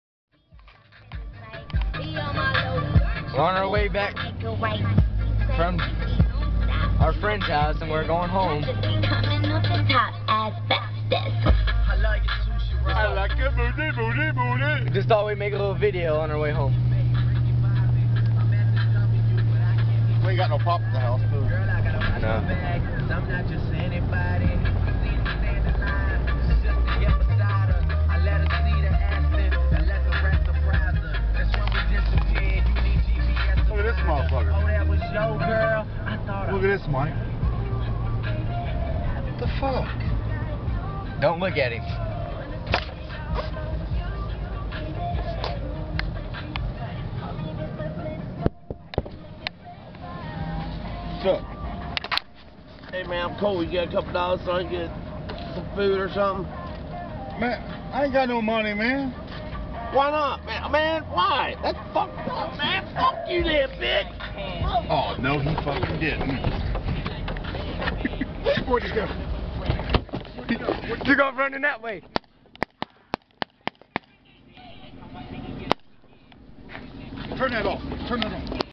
Bum
Me shooting at a bum with a paintball gun